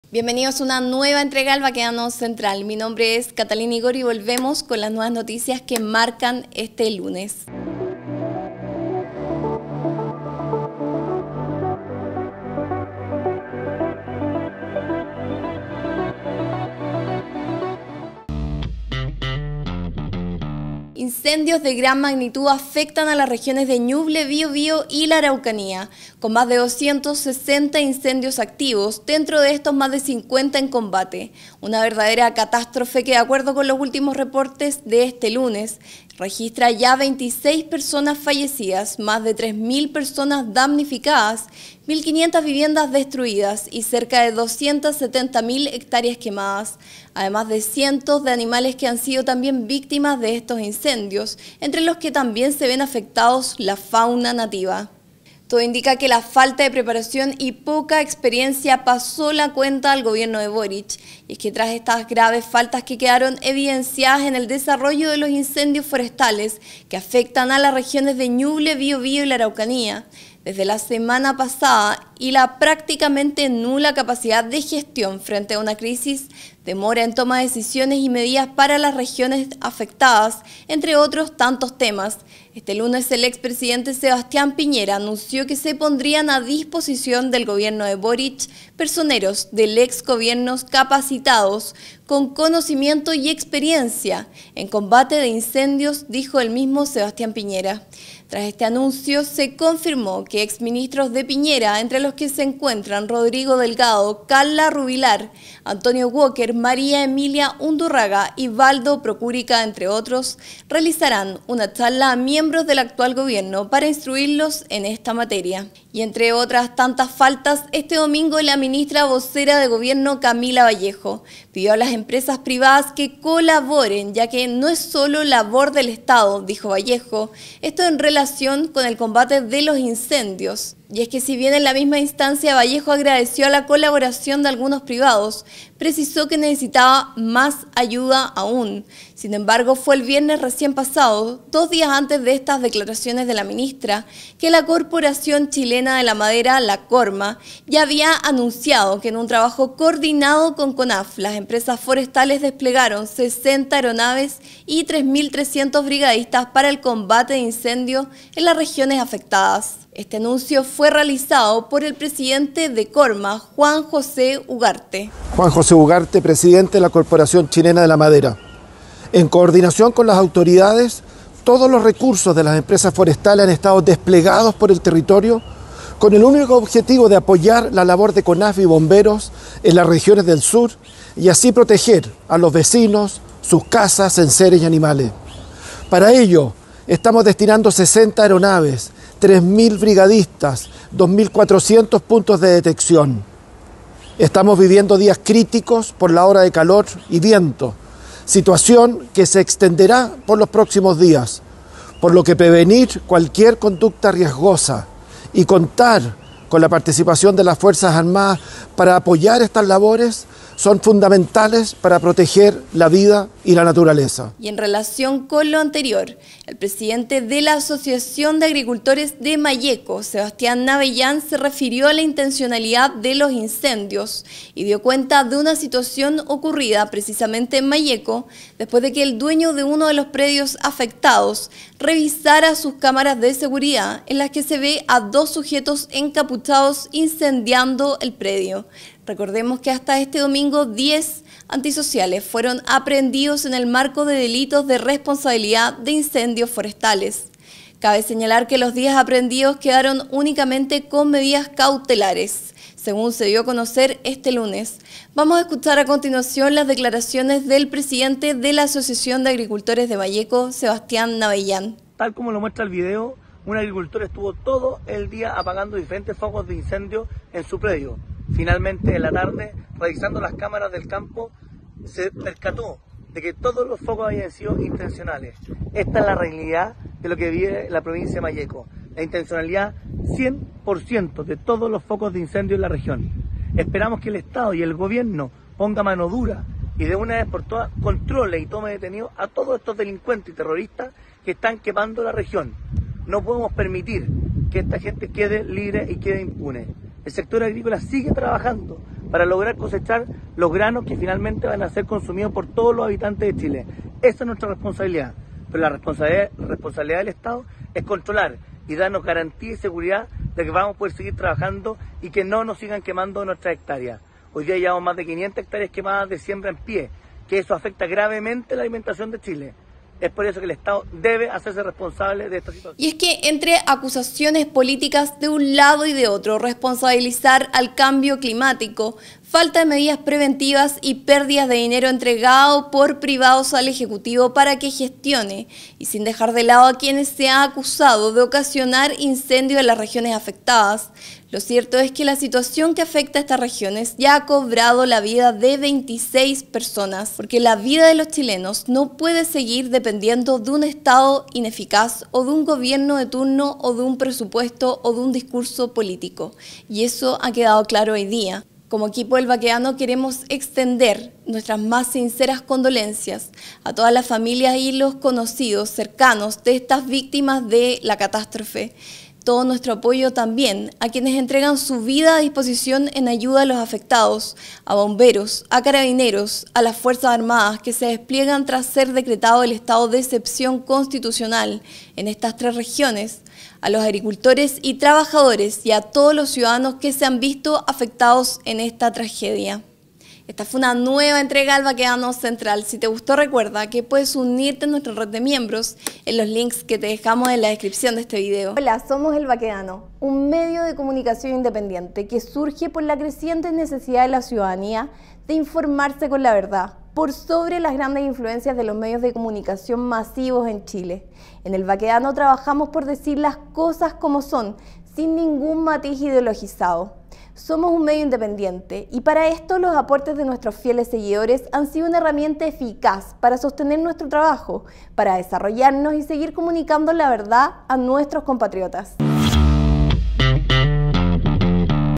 Especial incendios: Denuncian intencionalidad, Piñera instruye a Boric y desmienten a Vallejo
Haz tu aporte y así nos ayudarás a seguir con el periodismo independiente.

¡COMPRA TU TAZÓN AQUÍ!

Medio de comunicación independiente.